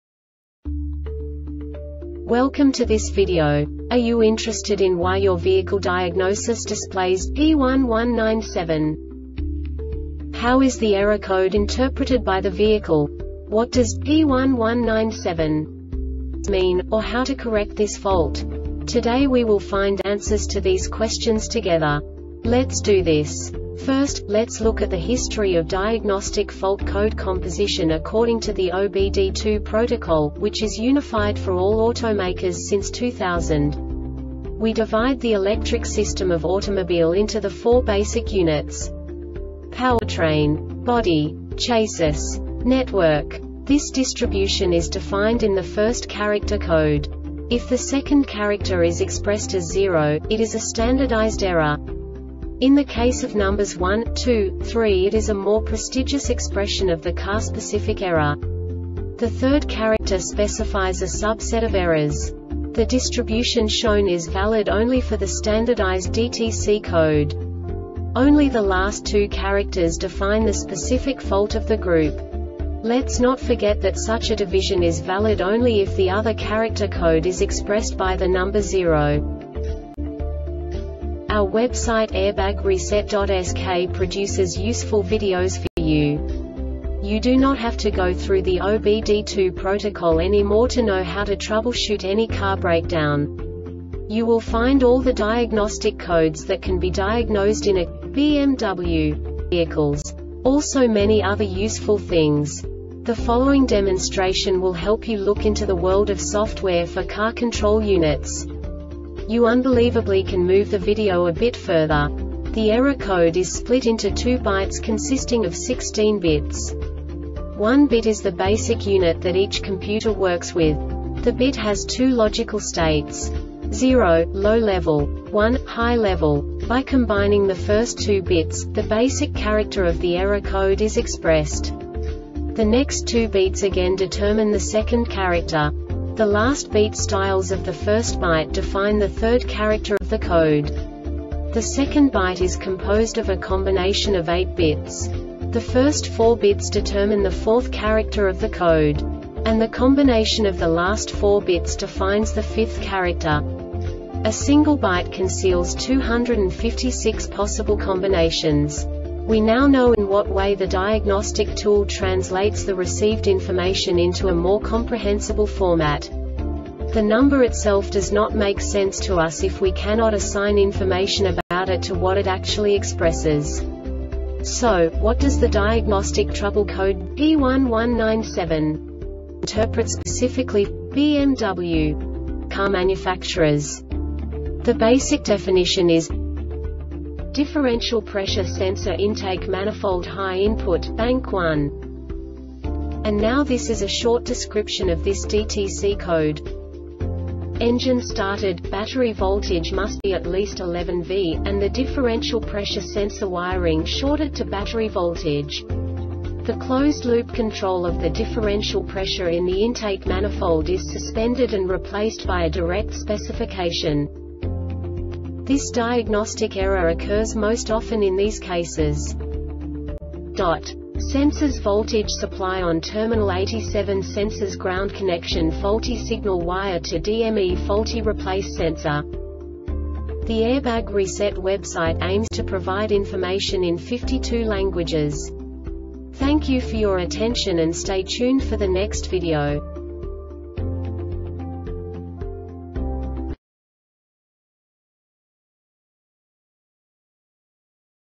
DTC Hyundai P1197 Short Explanation
Contents:
0:21 Basic DTC analysis according to OBD2 protocol standard.
1:48 Insight into programming
2:58 A diagnostically understandable description of the Diagnostic Trouble Code
3:05 Basic error definition

Make
BMW
Code
P1197
Definition
Differential Pressure Sensor Intake Manifold High Input Bank 1
Description
Engine started, battery voltage must be at least 11v, and the differential pressure sensor wiring shorted to battery voltage The closed loop control of the differential pressure in the intake manifold is suspended and replaced by a direct specification
Cause
Sensors voltage supply on Terminal 87
Sensors ground connection faulty
Signal wire to DME faulty
Replace sensor